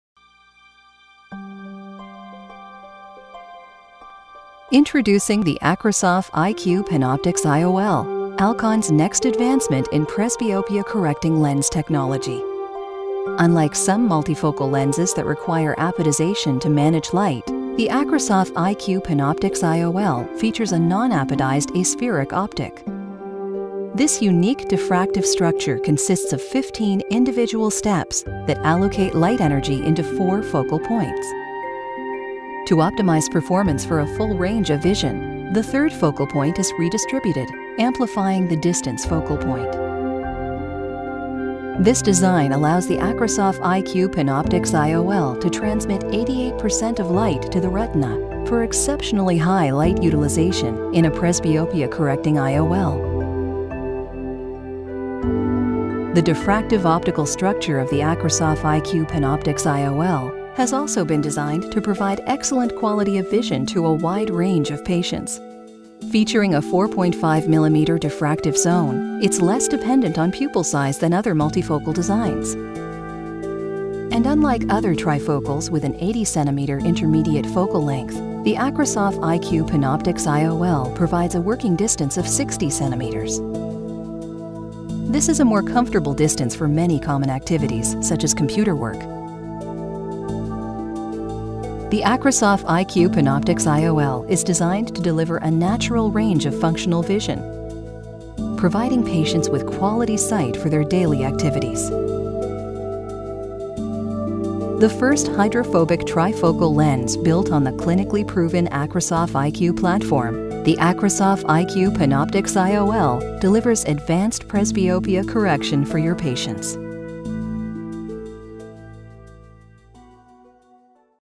PanOptix® IOL
The PanOptix IOL (Intraocular Lens) is designed and manufactured by Alcon and is the first trifocal lens to be FDA approved in the United States. The lens is designed to not only get rid of the cataract but is also designed to bend light rays to allow light to focus on the retina (the back surface of the eye) to provide for improved distance, intermediate, and near vision.